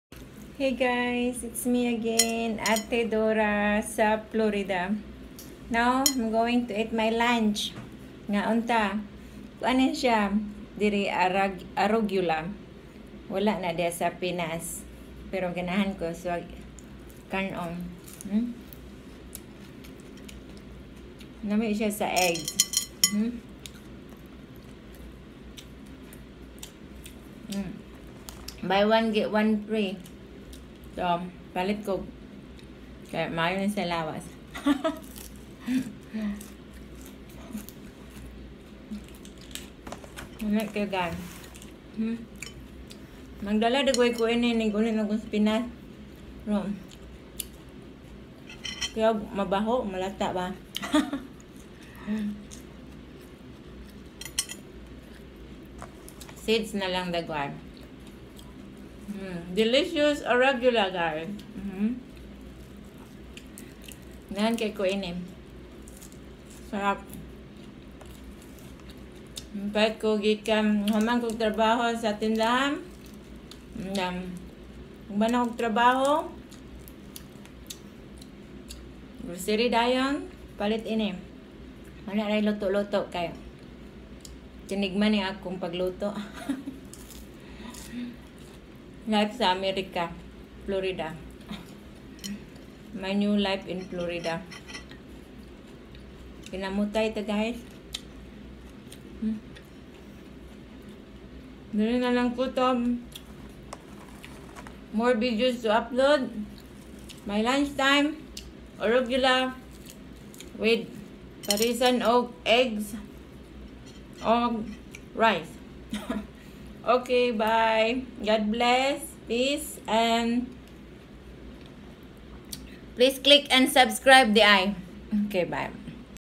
June 30, 2019 eating Arugula leaves
Ate Dora eating Arugula